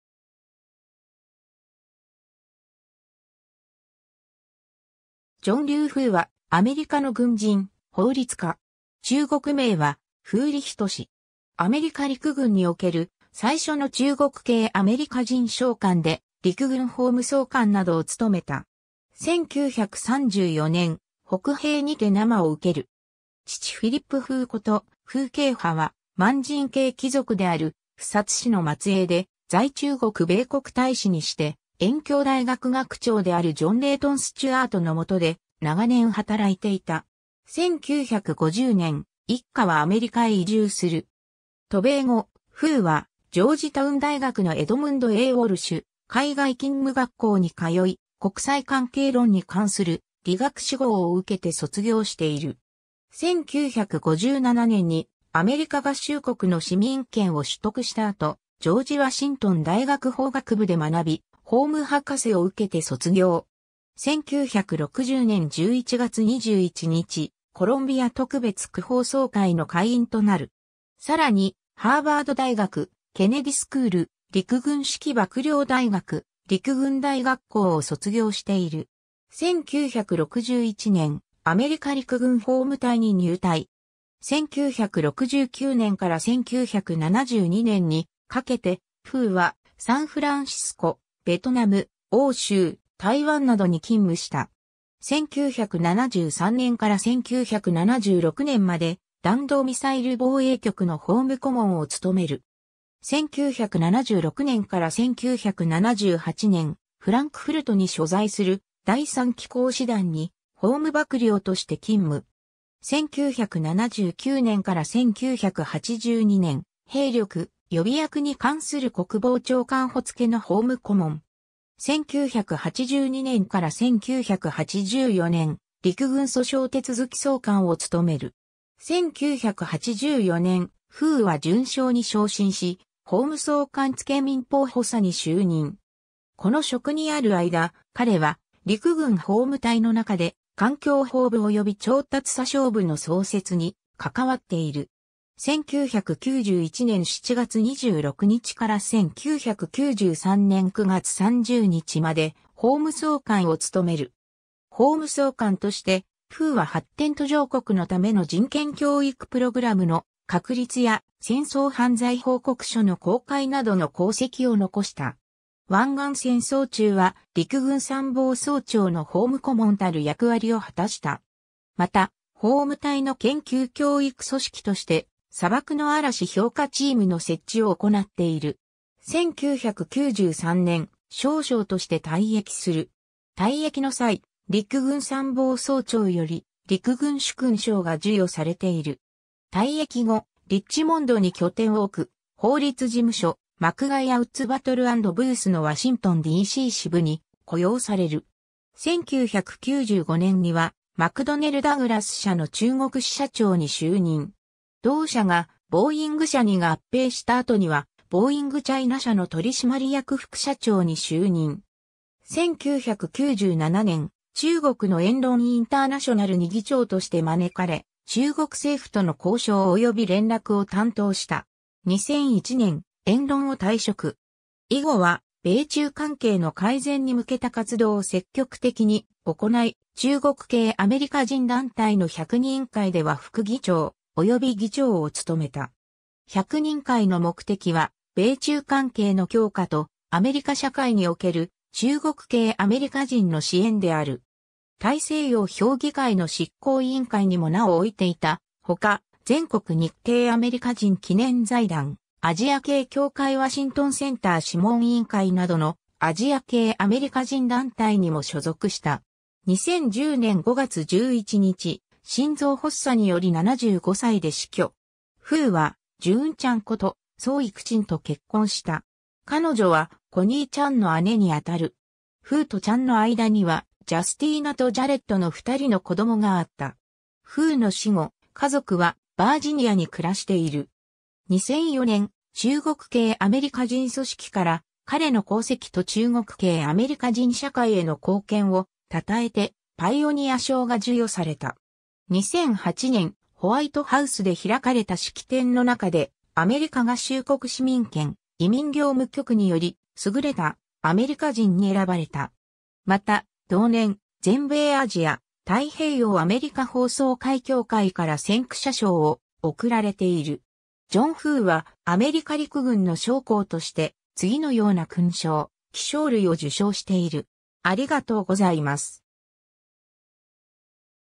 ジョン・フー
ジョン・リュー・フー（John Liu Fugh, 1934年9月12日 - 2010年5月11日 ）は、アメリカの軍人、法律家。
中国名は傅履仁（フー・リュージェン、ふ・りじん）。
アメリカ陸軍における最初の中国系アメリカ人将官で、陸軍法務総監(Judge Advocate General of the United States Army)などを務めた。
 1934年、北平（現北京市）にて生を受ける。
父フィリップ・フー (Philip Fugh) こと傅涇波（フー・ジンボー、ふ・じんは）は満人系貴族である富察氏の末裔で、在中国米国大使にして燕京大学学長であるジョン・レイトン・スチュアートの元で長年働いていた。
1950年、一家はアメリカへ移住する。
 渡米後、フーはジョージタウン大学のエドムンド・A・ウォルシュ海外勤務学校に通い、国際関係論に関する理学士号(Bachelor of Science)を受けて卒業している。
1957年にアメリカ合衆国の市民権を取得した後、ジョージ・ワシントン大学法学部で学び、法務博士(Juris Doctor)を受けて卒業。
 1960年11月21日、コロンビア特別区法曹会(District of Columbia Bar)の会員となる。
さらにハーバード大学ケネディスクール、陸軍指揮幕僚大学、陸軍大学校を卒業している。
 1961年、アメリカ陸軍法務隊(JAG)に入隊。
1969年から1972年にかけて、フーはサンフランシスコ、ベトナム、欧州、台湾などに勤務した。
1973年から1976年まで弾道ミサイル防衛局(Ballistic Missile Defense Office)の法務顧問を務める。
1976年から1978年、フランクフルトに所在する第3機甲師団に法務幕僚(Staff Judge Advocate)として勤務。
1979年から1982年、兵力・予備役に関する国防長官補（Assistant Secretary of Defense (Manpower and Reserve Affairs)）付の法務顧問。
1982年から1984年、陸軍訴訟手続総監(Chief of Army Litigation)を務める。
 1984年、フーは准将に昇進し、法務総監付民法補佐(Assistant Judge Advocate General for Civil Law)に就任。
この職にある間、彼は陸軍法務隊の中で環境法部および調達詐称部の創設に関わっている。
 1991年7月26日から1993年9月30日まで法務総監(The Judge Advocate General, TJAG)を務める。
法務総監として、フーは発展途上国の為の人権教育プログラムの確立や戦争犯罪報告書の公開などの功績を残した。
湾岸戦争中は陸軍参謀総長の法務顧問たる役割を果たした。
また、法務隊の研究教育組織として、砂漠の嵐評価チーム(Desert Storm Assessment Team)の設置を行なっている。
 1993年、少将として退役する。
退役の際、陸軍参謀総長より陸軍殊勲章が授与されている。
 退役後、リッチモンドに拠点を置く法律事務所マクガイア・ウッズ・バトル・アンド・ブースのワシントンDC支部に雇用される。
1995年にはマクドネル・ダグラス社の中国支社長に就任。
同社がボーイング社に合併した後にはボーイング・チャイナ社(Boeing China, Inc.)の取締役副社長(Executive Vice President)に就任。
1997年、中国のエンロン・インターナショナルに議長(Chairman)として招かれ、中国政府との交渉および連絡を担当した。
 2001年、エンロンを退職。
以後は米中関係の改善に向けた活動を積極的に行い、中国系アメリカ人団体の百人会では副議長および議長を務めた。
百人会の目的は、米中関係の強化とアメリカ社会における中国系アメリカ人の支援である。
大西洋評議会の執行委員会にも名を置いていたほか、全国日系アメリカ人記念財団、アジア系協会ワシントンセンター諮問委員会などのアジア系アメリカ人団体にも所属した。
2010年5月11日、心臓発作により75歳で死去。
 フーはジューン・チャン(June Chung)こ...